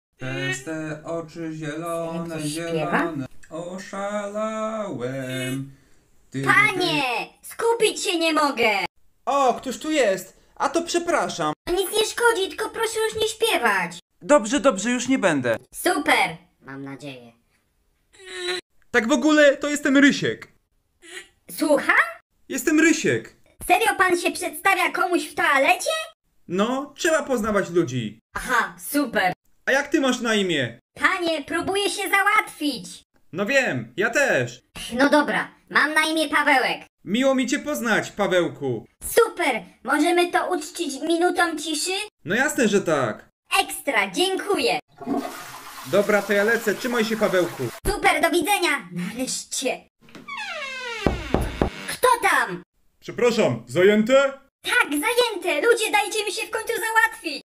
POV: Publiczna toaleta 2
Pawełek nigdy nie będzie miał spokoju w toalecie